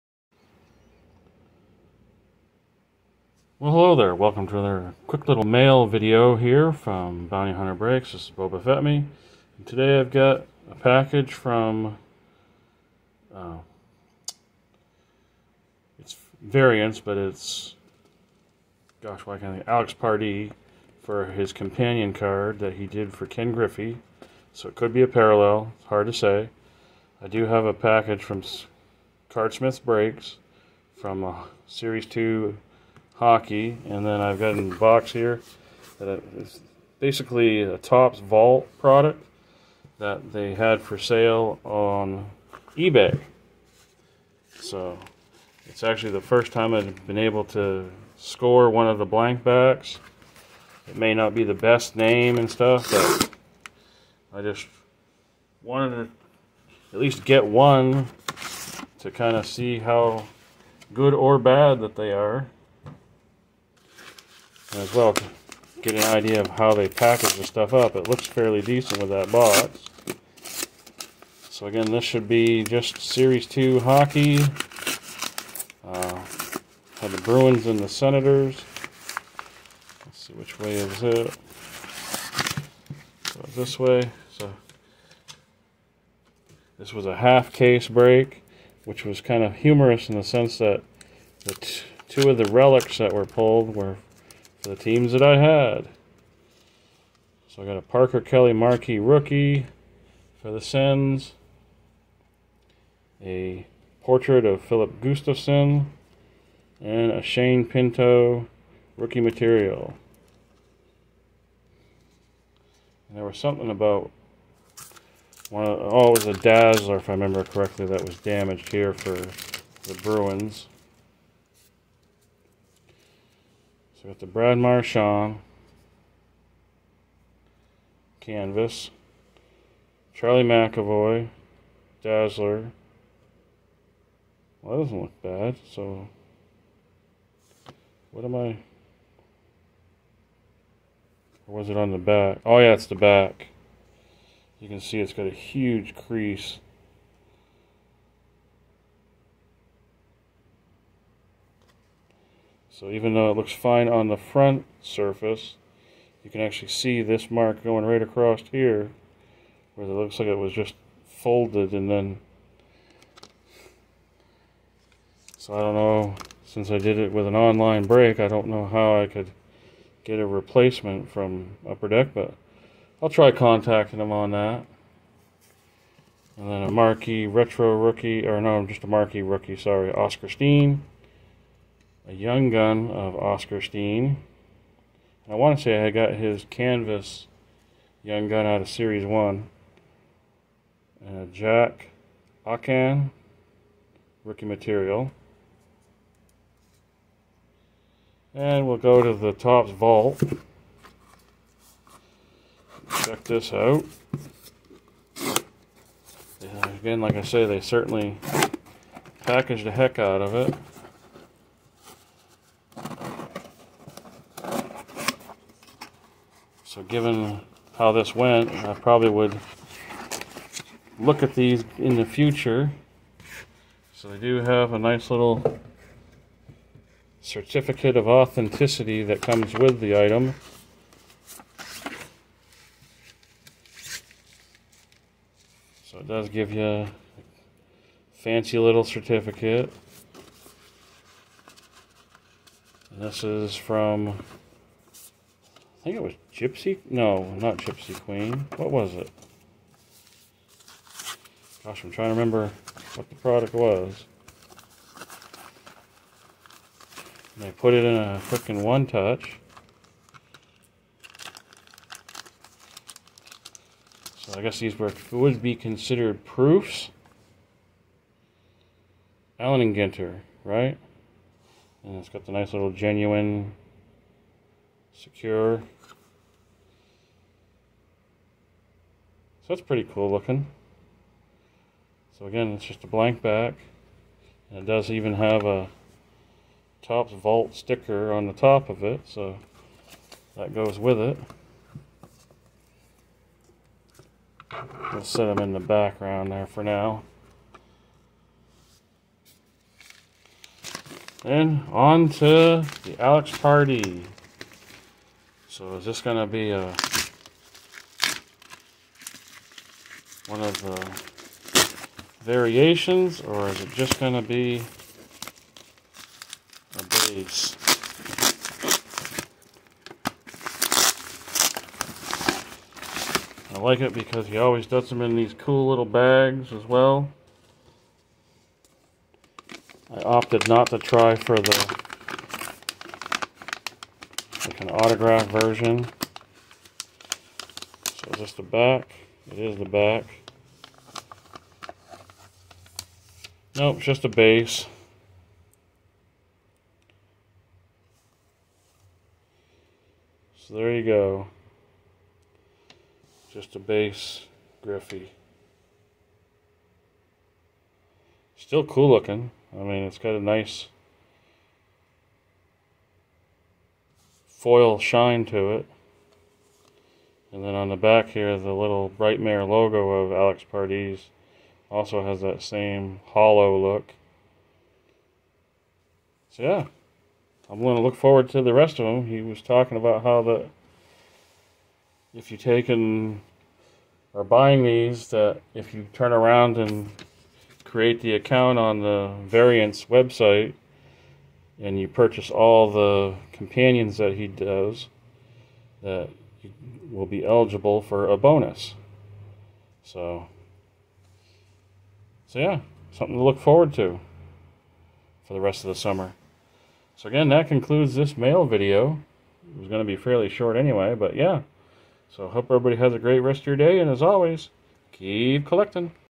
6-26-2022 Mail Day CSB, Alex Pardee and Topps Vault
Quick mail day video revealing latest companion card from Alex Pardee, pickup from Topps Vault and some UD Series 2 from half case break of Series 2 Hockey.